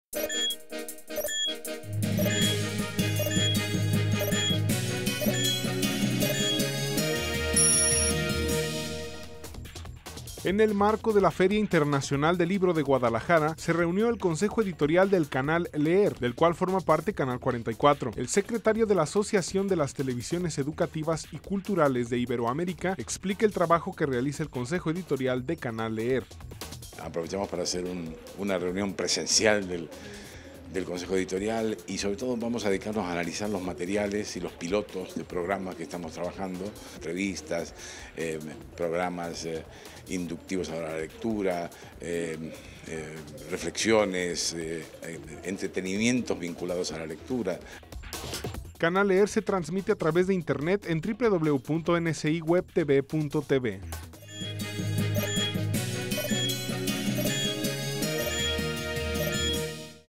Corte 44 / Prepara Canal Leer nuevos contenidos para 2013 para fomentar lectura en Iberoamérica
Corte 44 -- En el marco de la Feria Internacional del Libro de Guadalajara,  se reunió el consejo editorial del Canal Leer, del cual forma parte Canal 44.
El secretario de la Asociación de las Televisiones Educativas y Culturales de Iberoamérica, explica el trabajo que realiza el consejo editorial de Canal Leer.
Usted puede ver la oferta cultural de Canal Leer a través de internet